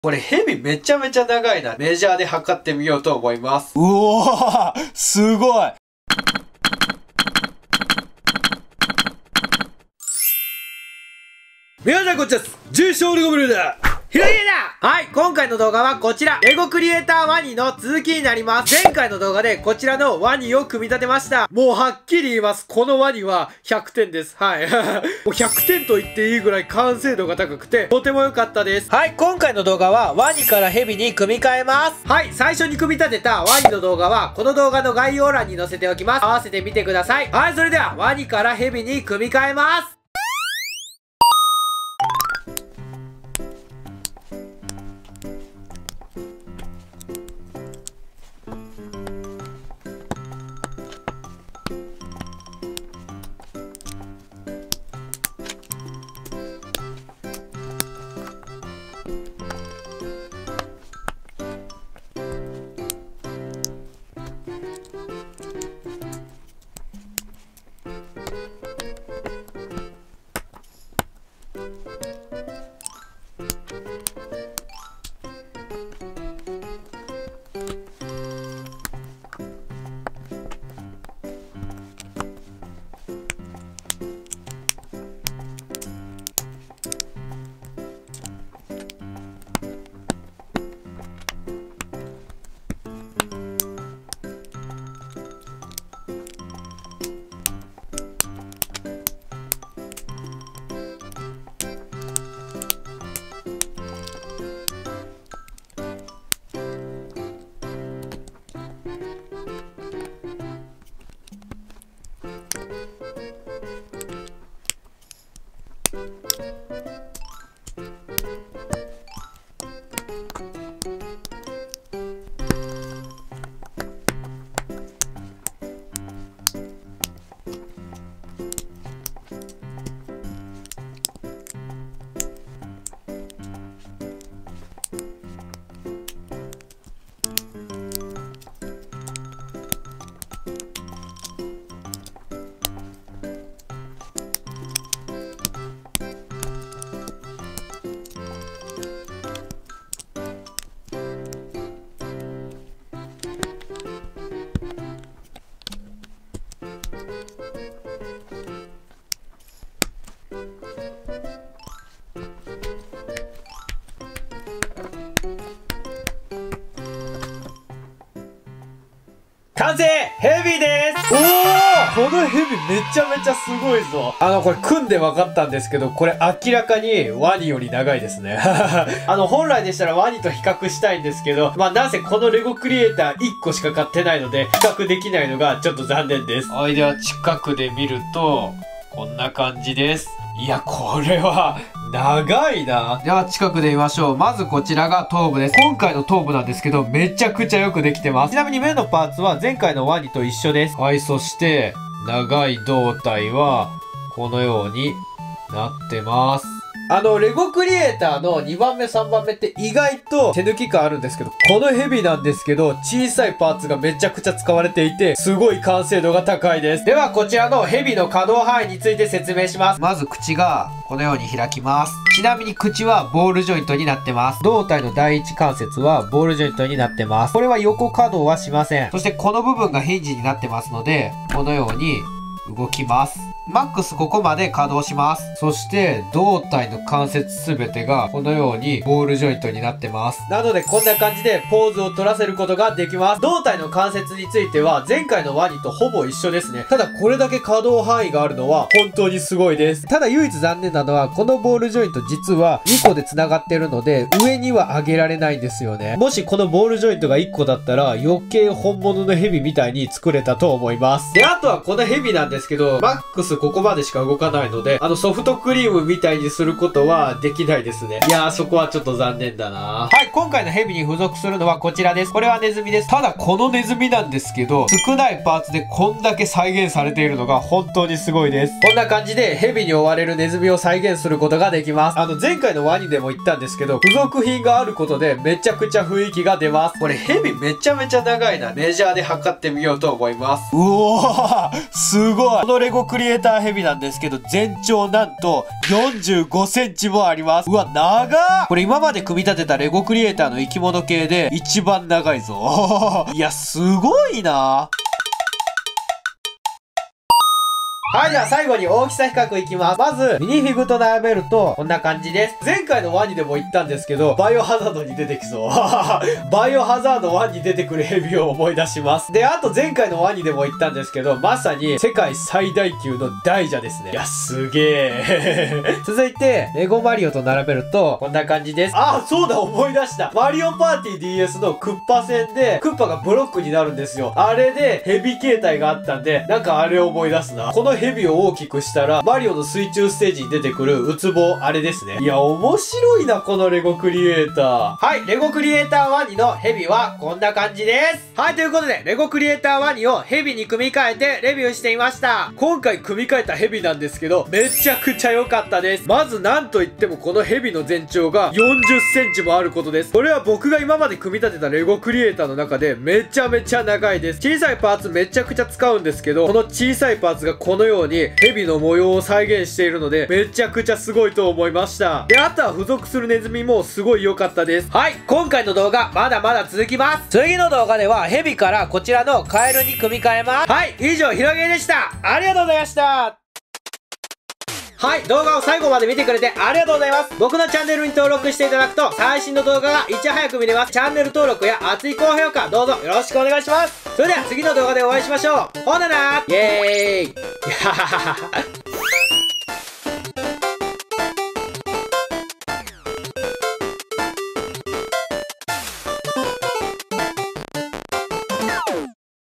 【全長○○0㎜はかなり長い！！】レゴクリエイター ワニ ヘビ編 31121 [2/3] Lego Creator Crocodile Snake Edition
最初に組み立てたワニが凄すぎたので、今回のヘビもワニに負けないくらい凄いものなのでは！？